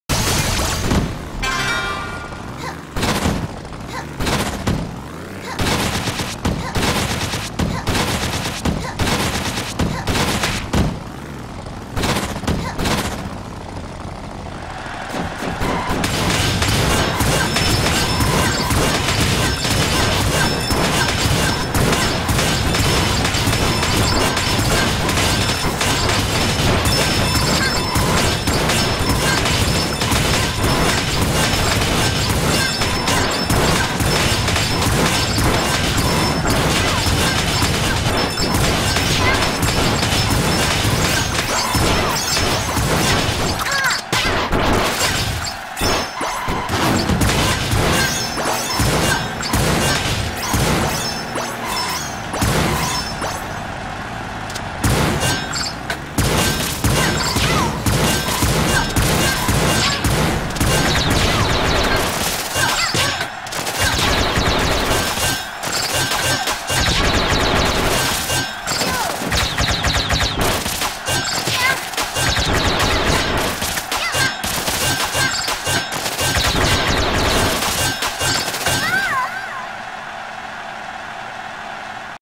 PSA: Do not go over 4 double jumps for the Spade quick kill
Just a short video to show new runners what happens if they overshoot the quick kill.